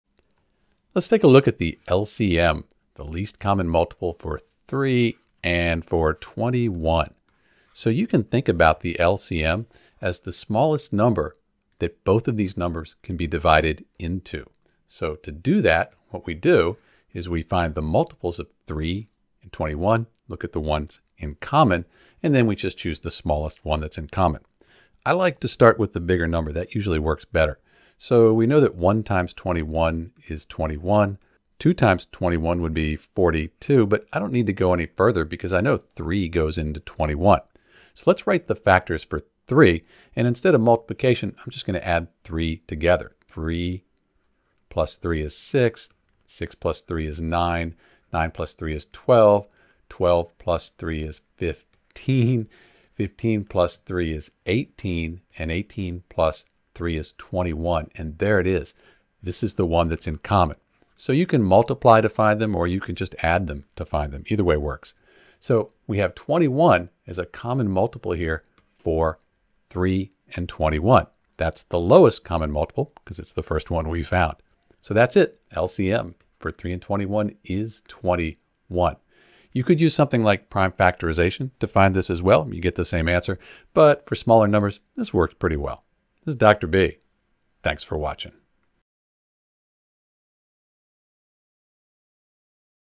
Find the Least Common Multiple (LCM) for 3 and 21
To find the least common multiple (LCM) for 3 and 21, we need to find the smallest number that is a multiple of both 3 and 21. That means the smallest number that both 3 and 21 go into evenly.

Step-by-step description of how to find the LCM for 3 and 21:

Step 1: List the Multiples of Each Number
• For 3: 3, 6, 9, 12, 15, 18, 21, 24, ...
• For 21: 21, 42, 63, 84, 105, 126, ...

Step 2: Identify the Common Multiples
• Look at the lists of multiples for both 3 and 21. The first common multiple is 21.

Step 3: Determine the LCM
• The LCM is the smallest common multiple. In this case, 21 is the smallest common multiple, so it is the LCM of 3 and 21.

So, the LCM of 3 and 21 is 21. That means 21 is the smallest number that is divisible by both 3 and 21 without leaving a remainder.